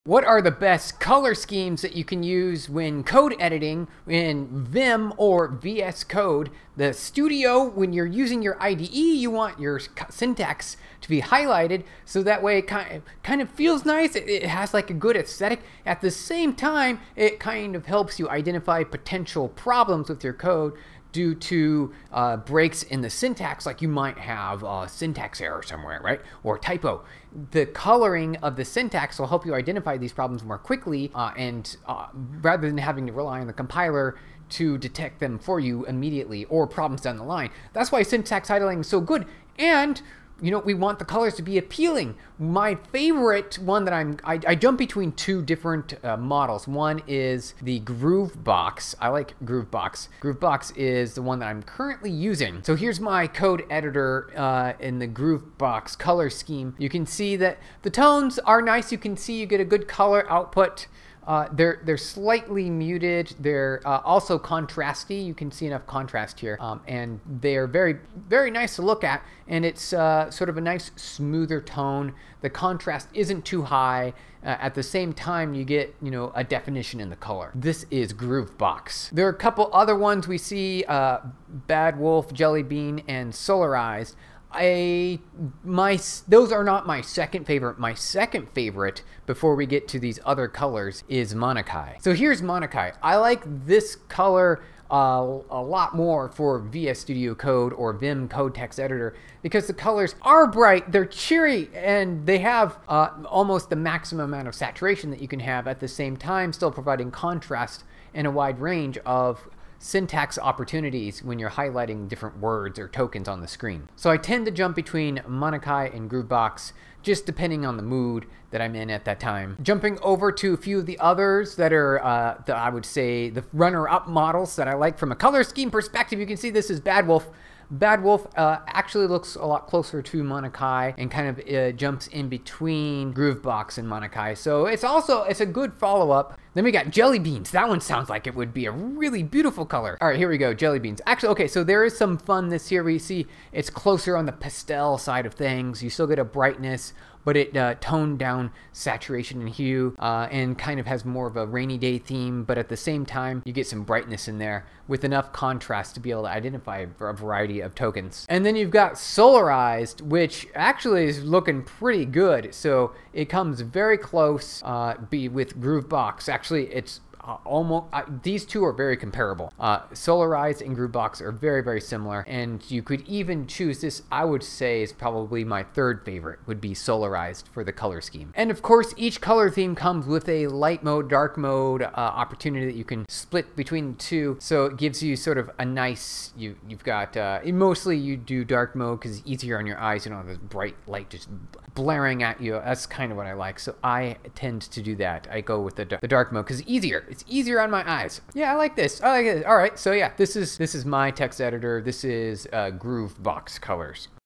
Best Color Theme for Vim and Visual Studio Code Editor for Syntax Highlighting
What's the best color scheme for coding in text editors like Vim or VS Code? It's important to have highlighted syntax in your Integrated Development Environment (IDE). Good Syntax highlighting not only looks nice but it also helps you quickly spot problems with your code. It saves time since you don't have to rely on your compiler to identify these issues.

Now, let's talk about my favorite color themes. I switch between two: Gruvbox and Monokai. Gruvbox offers slightly muted and contrasting colors, which are nice and smooth to the eye.

The contrast isn't too high, but you can easily differentiate the colors. Monokai is another brilliant theme with bright, cheery colors that are highly saturated. It still provides contrast while highlighting different text on the screen.

Depending on my mood, I tend to switch between these two. Other color schemes I favor are BadWolf, Jellybean, and Solarized. Bad Wolf shares similarities with both Gruvbox and Monokai, making it a good alternative.

Jellybean showcases pastel shades and subdued saturation with a hint of brightness, great for a variety of coding tokens. Lastly, we have Solarized, bearing close resemblance to Gruvbox. It is also attractive, making it my third favorite.